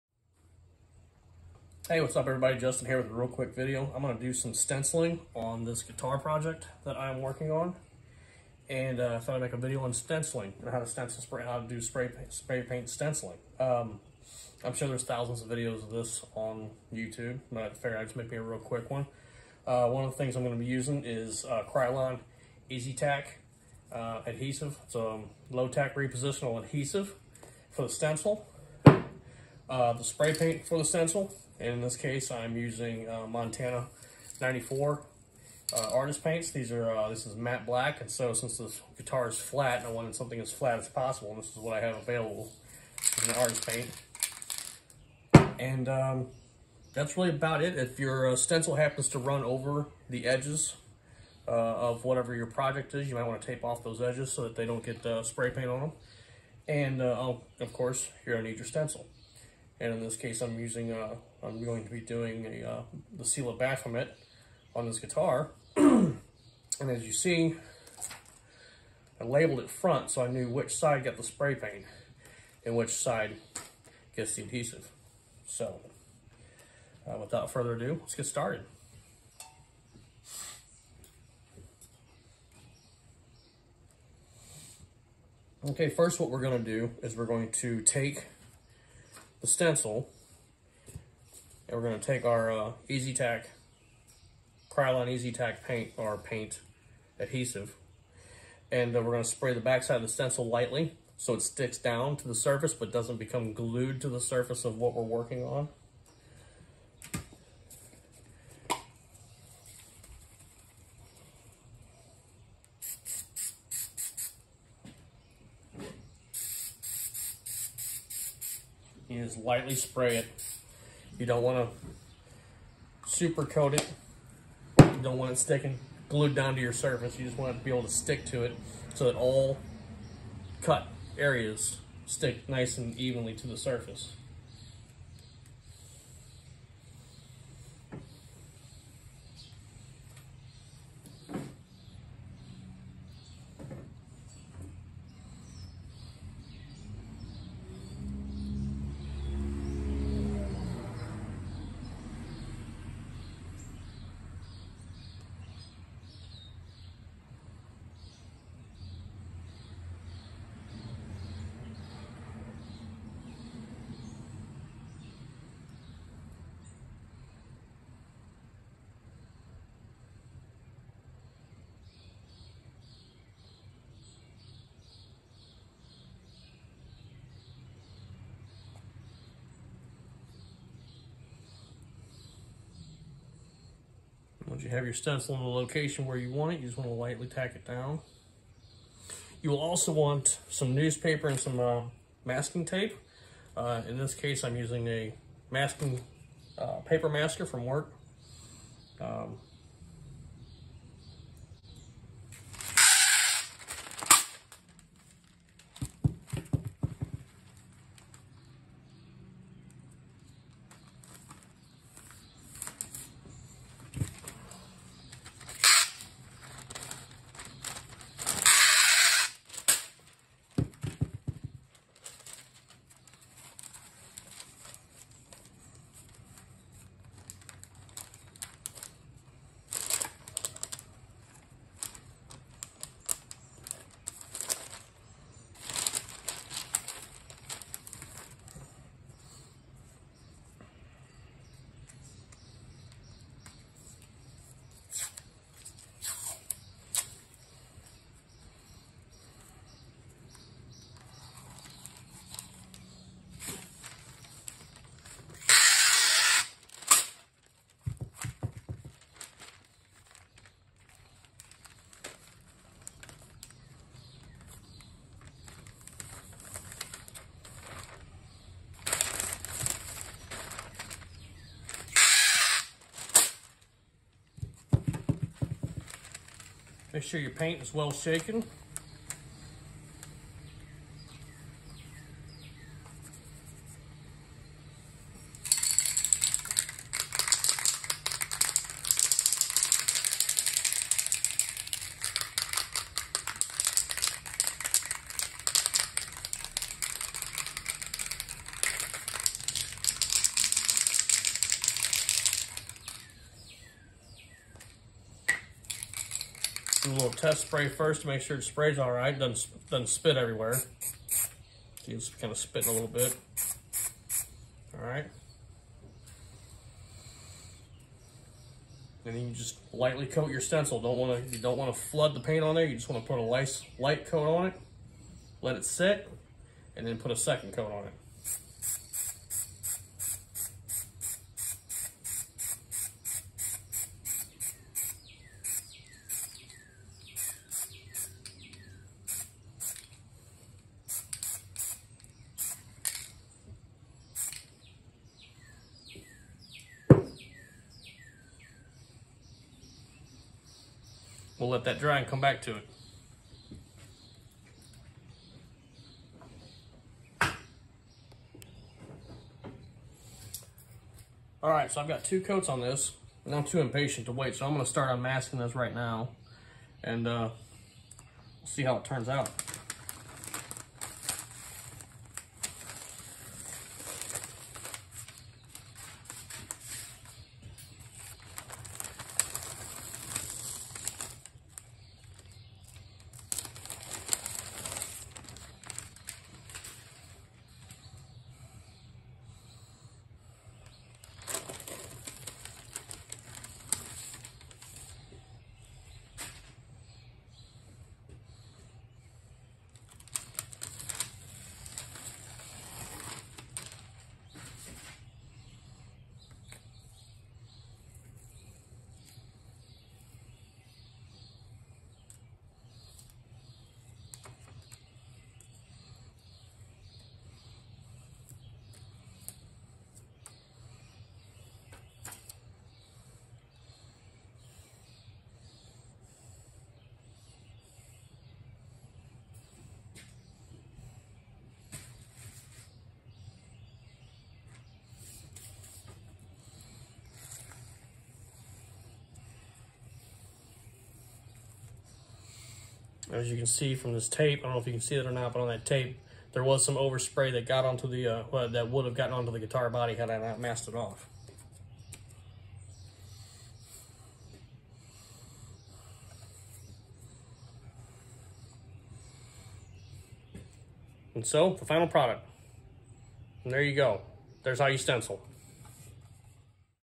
Guitar Stencil
This is a quick tutorial on how to spray paint a stencil and get clean edges with little to no overspray.
 This is done on a guitar body that has been almost a year in the making.